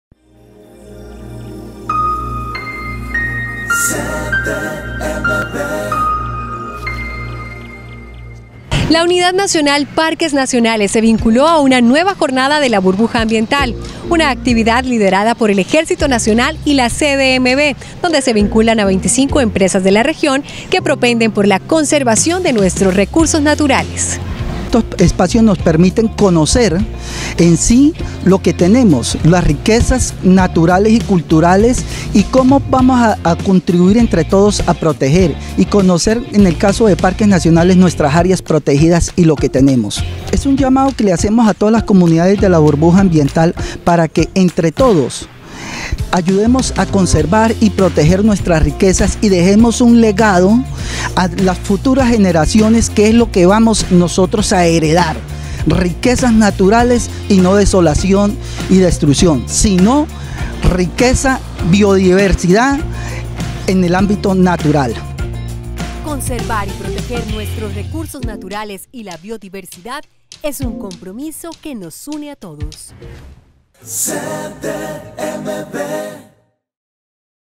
Parques Nacionales estuvo en la Burbuja Ambiental
BurbujaAmbiental Parques Nacionales se dirigió a todos los funcionarios que hacen parte de la Burbuja para explicar y conocer la importancia de las áreas protegidas y parques naturales. Veamos